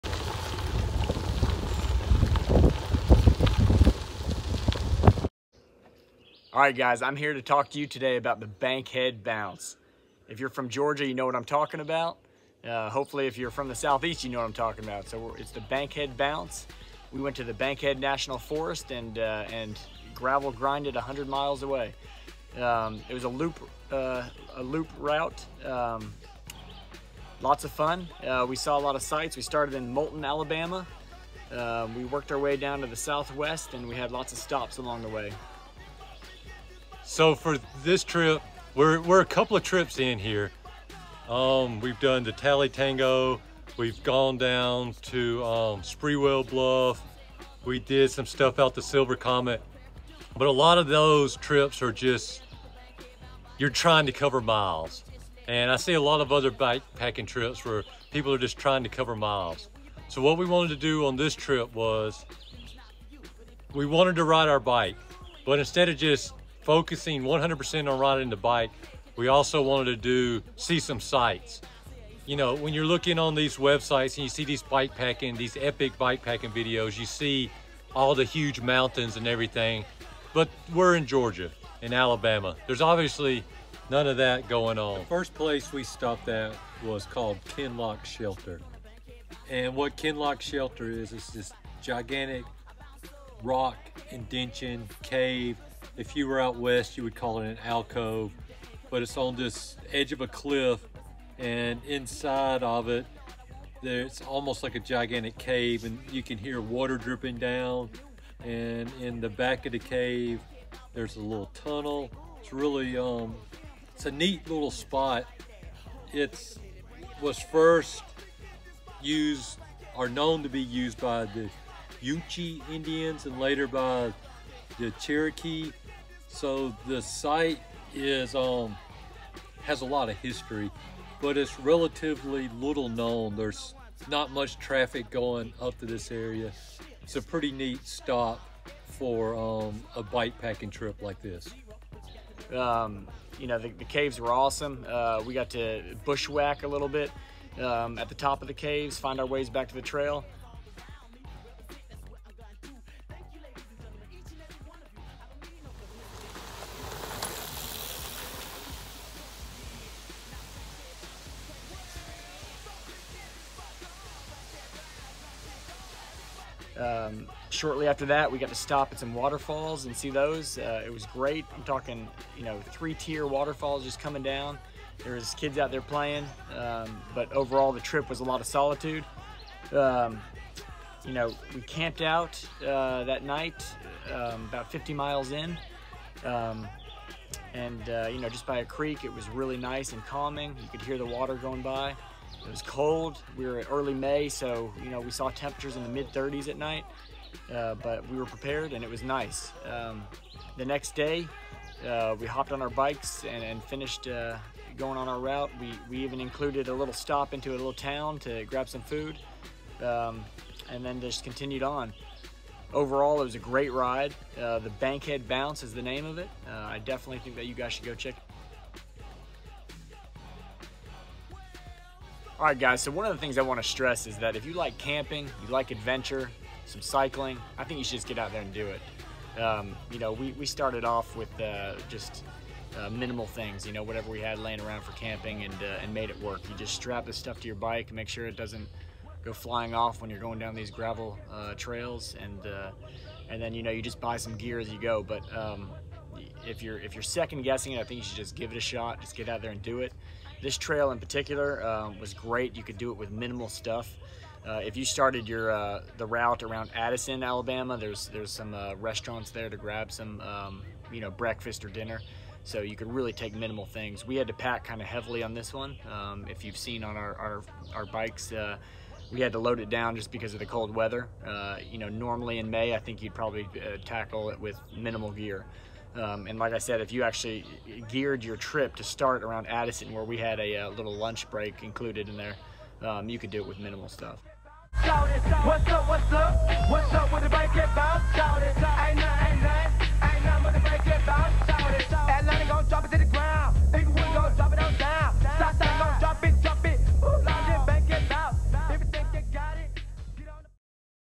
Bikepacking the Bankhead National Forest in Northeast Alabama with Specialized Diverge
The Bankhead National Forest is a wonderland of natural beauty.  Waterfalls, rivers, cliffs, open farm land, what more could you ask for on a bikepacking trip in the Southeast?  Lots of roads with some of Alabama’s smoothest gravel.  Our adventure to Kenlock shelter and Kenlock waterfall is only a small sample of what the area has to offer.